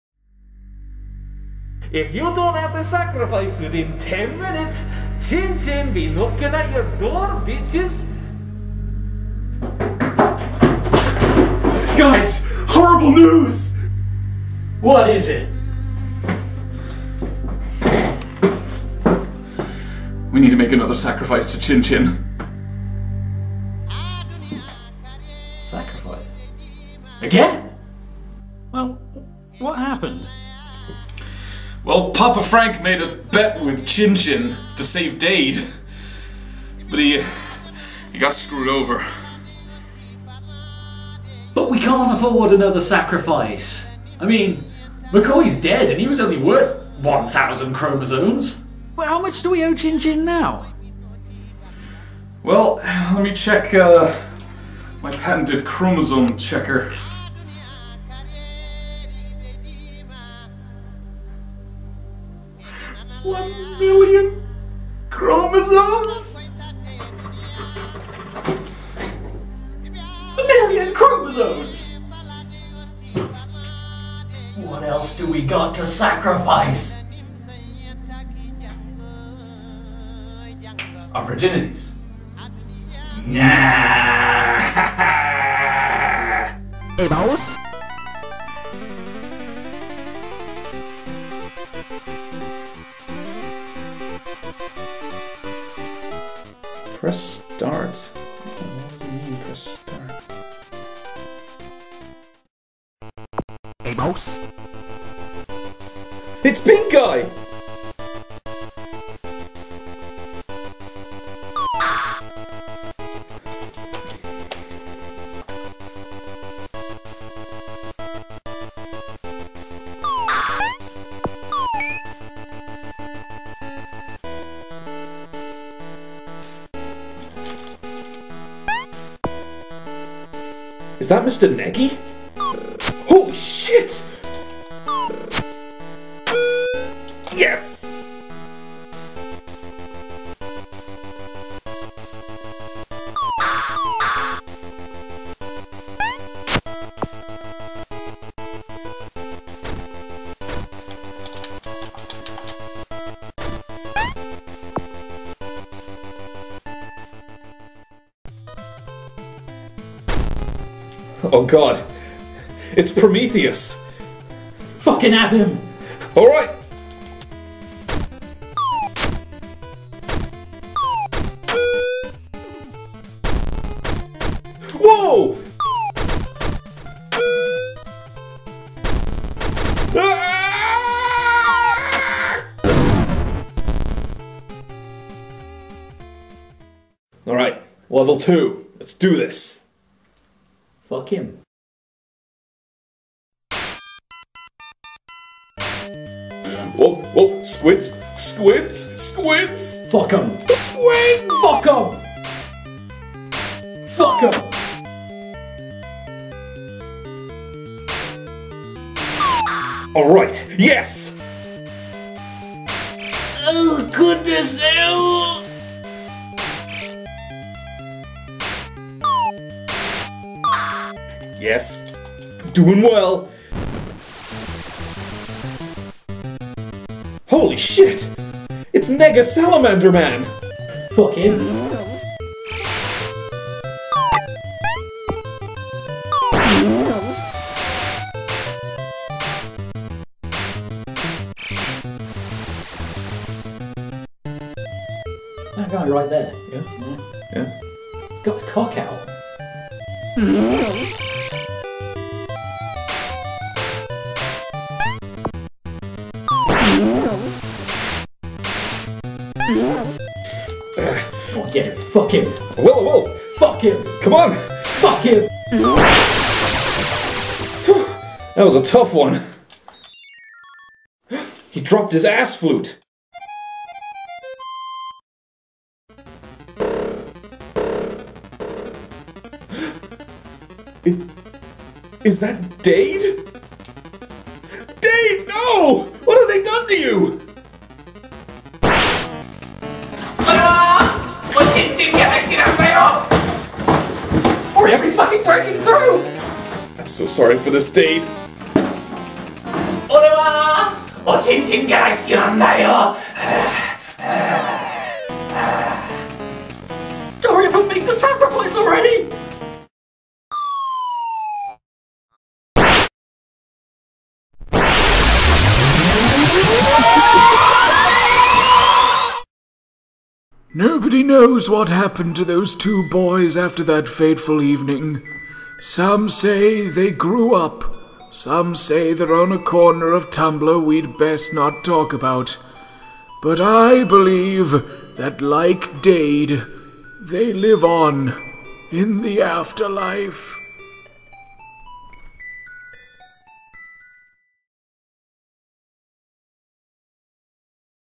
CHIN CHIN SACRIFICE 2015 - Pink Guy Land (Gameboy)
This, I have worked on since day one of the sacrifice announcement. And now a few hours before the deadline, it is here! All pixel art in this video has been created by me. Of course, the music is ripped from various games.

I hope chin chin is pleased.